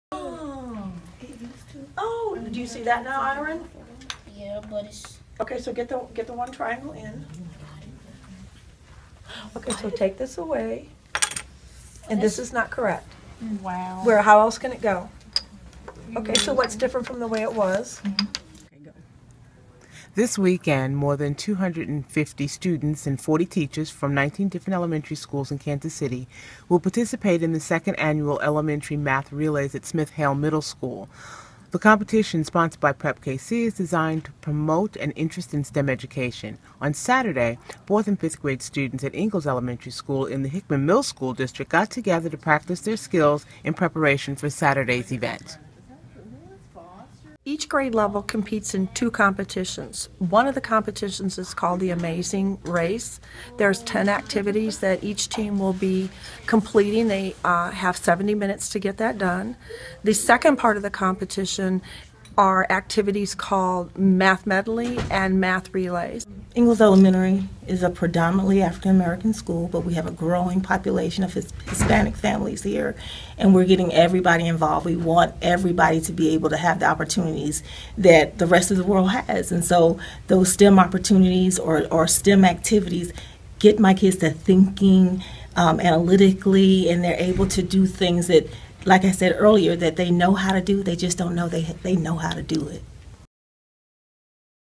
Math Relay Preparation
Students in Hickman Mills School District ready to compete in Prep KC's 2nd Annual Elementary School Math Relay competition